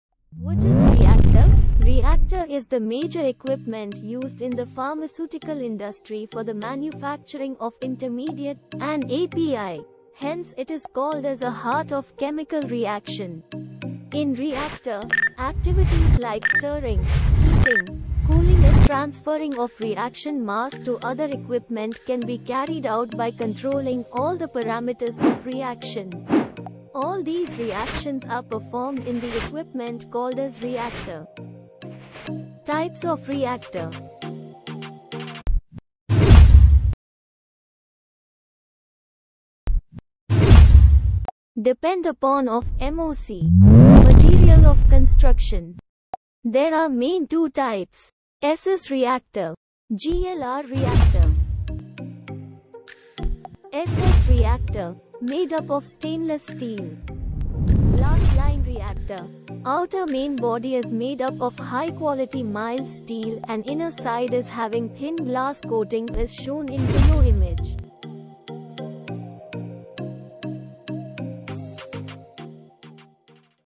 Types of Reactor || phrama chemical company
yah video Pharma API and chemical company me use kiye jane vale  equipment ( Reactor ) ke types ke bare he.